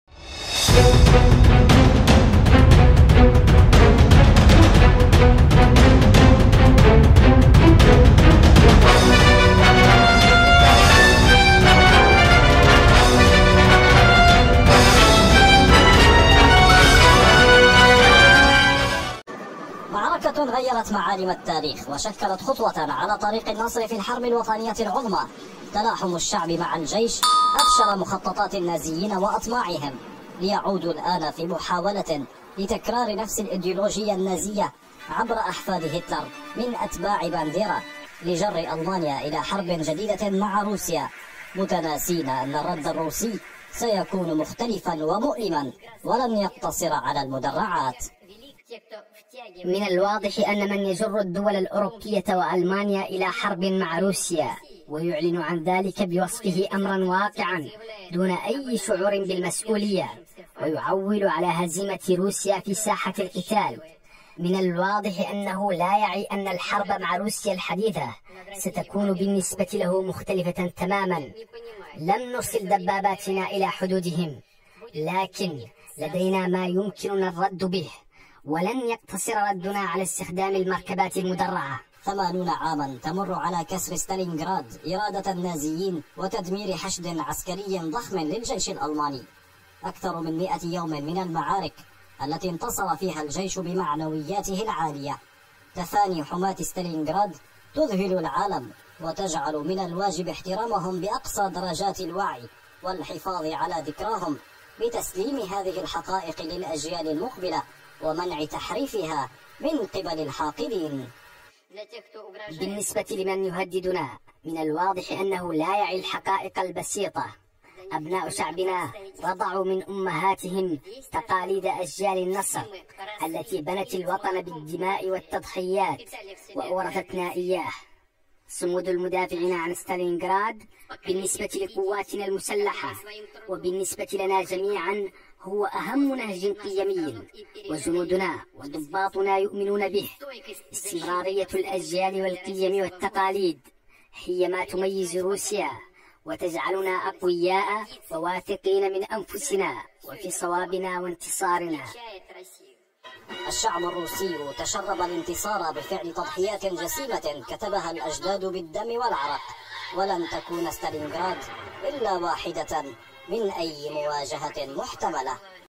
بوتين: على الجميع الفهم أن ردنا على التهديد لن يقتصر على المدرعات والحرب مع روسيا الحديثة😱
بوتين: على الجميع الفهم أن ردنا على التهديد لن يقتصر على المدرعات والحرب مع روسيا الحديثة ستكون مختلفة
الريال _السعودى
الجيش_المصري
أمريكا #
السعودية
روسيا #
روسيا_اوكرانيا
فلسطين
أبوظبي
Putin: Everyone should understand that our response to the threat will not be limited to armored vehicles, and the war with modern Russia will be different
Путин: Все должны понимать, что наш ответ на угрозу не ограничится бронетехникой, и война с современной Россией будет другой